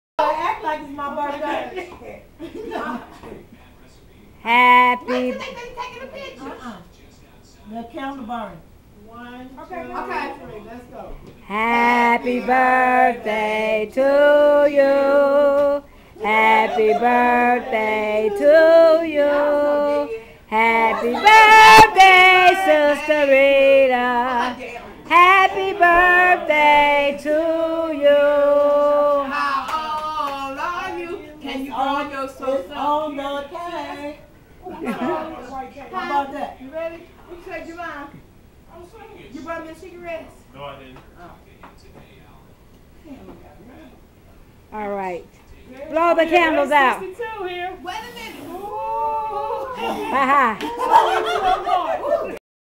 HAPPY BIRTHDAY SISTER P!!!
Sharon Adams Dinwiddie gave a Fish Fry at her house on Tuesday, June 12, 2012 and surprised Sister Rita Payton with an early birthday get together!